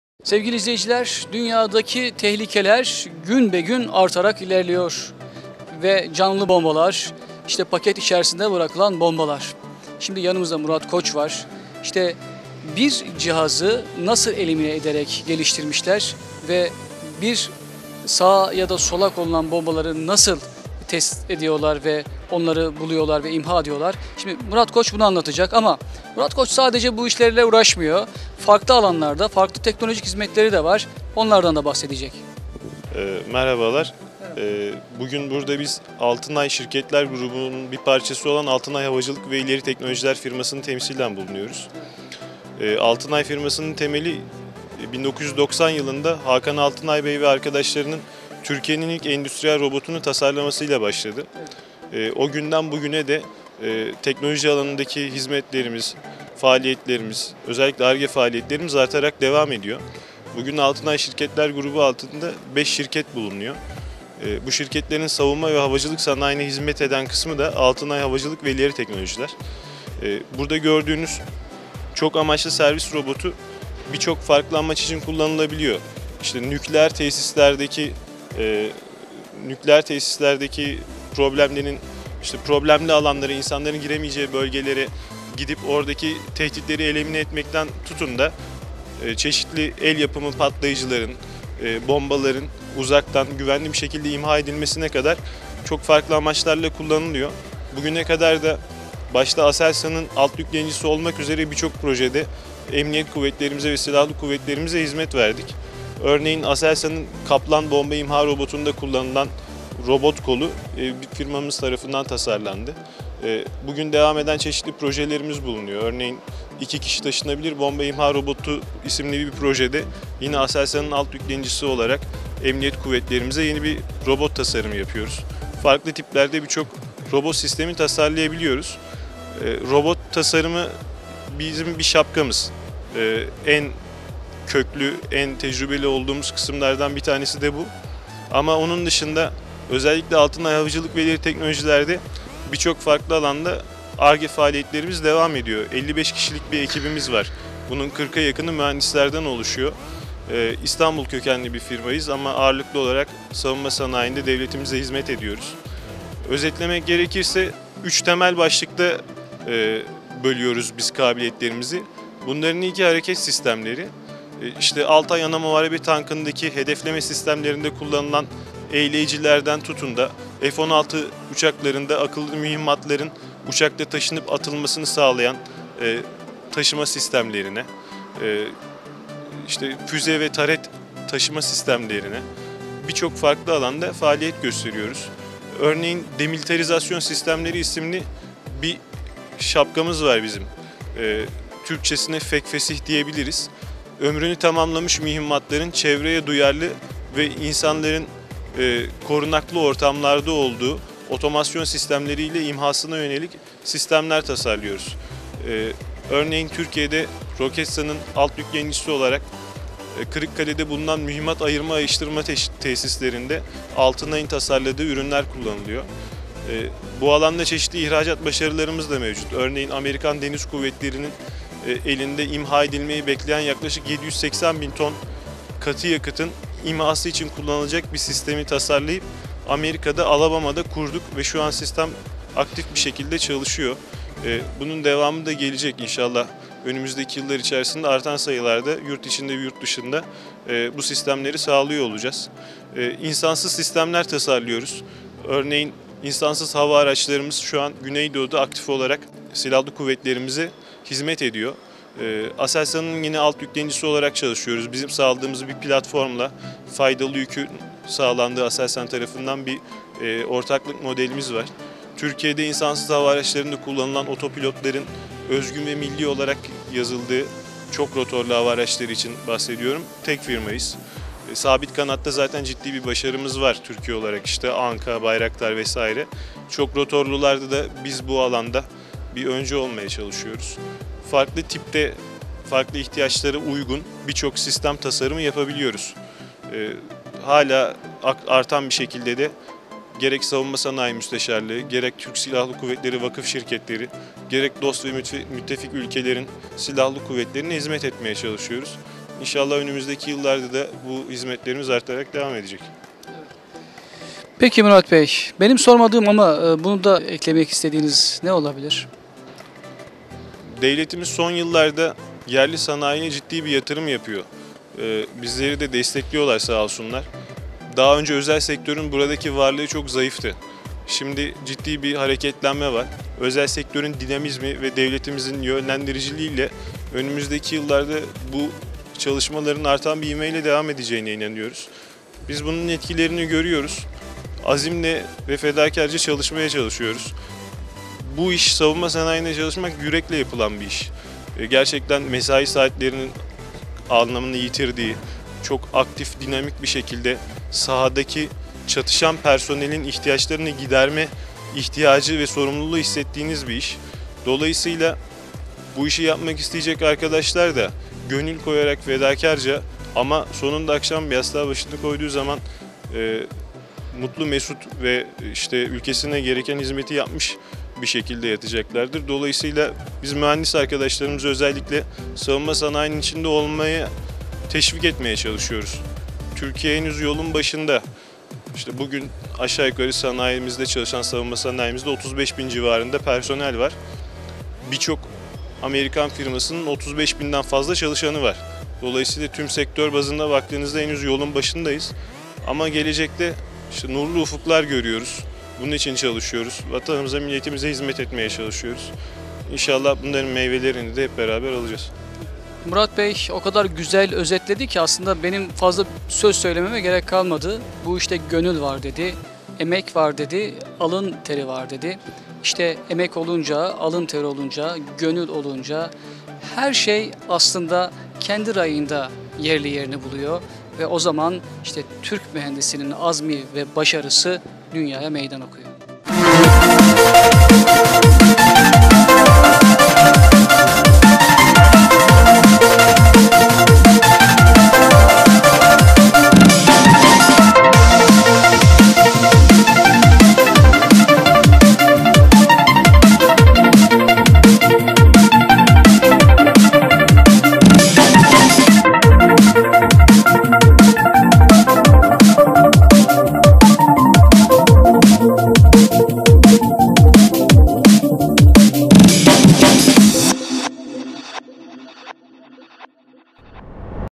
Hava Savunma Sanayi, Bombalar Nasıl İmha Ediliyor? - Teknovia
Robotlar Bombaları nasıl tespit edilip imha ediliyor?
Hava savunma sanayinde gelinen son nokta!